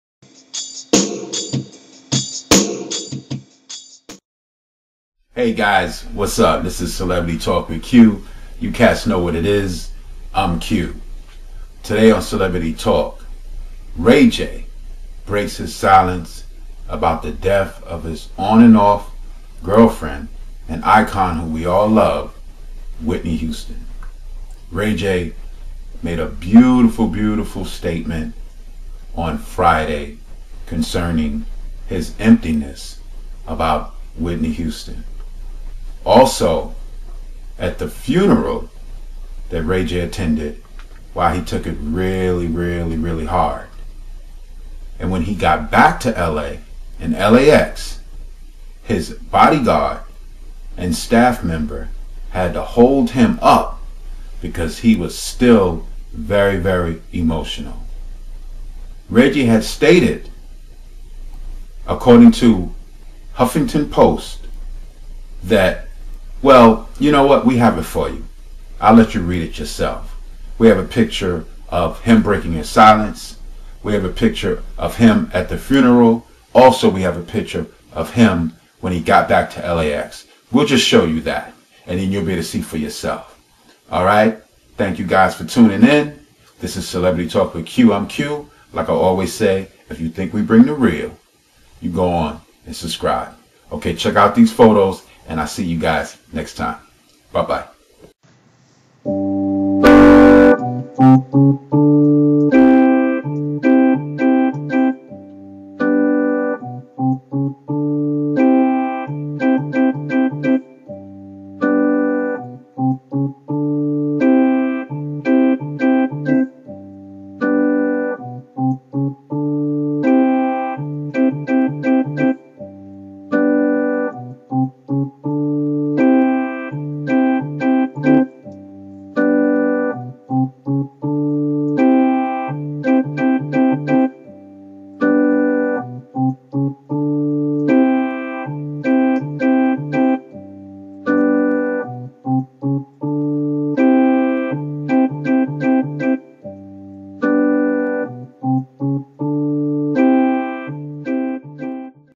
Whitney Houston: Ray J Opens Up About His Pain
According to Huffingtonpost.com, Ray J released a statment about Whitney Houston.  This was the first time he spoke about her death to the public.  He expressed sentimental words regarding his relationship with Whitney and the emptiness he feels inside now the she is gone.

Let us keep Ray J in our prayers as he deals with this trying time.

Sincerely,

TheSuperCelebs